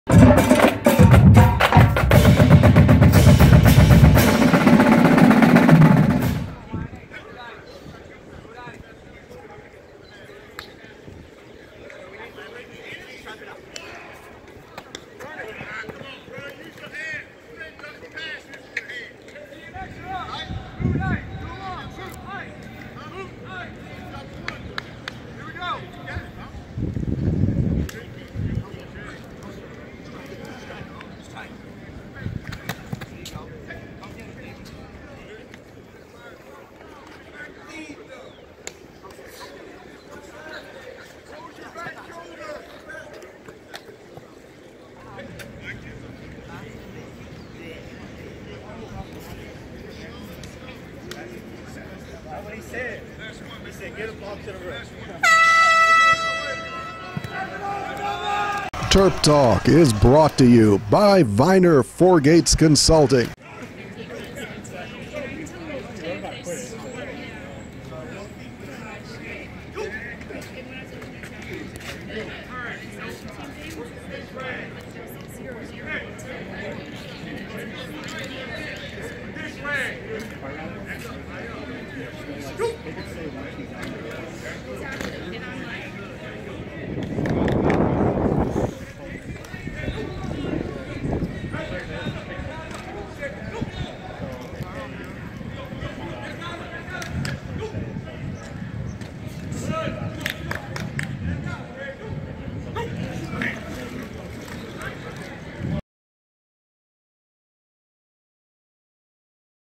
Maryland Football Spring Practice #9 QB's, D-backs + D-Line 2019 04 13
A full field look at practice #9 of spring football at Maryland Stadium / Byrd Stadium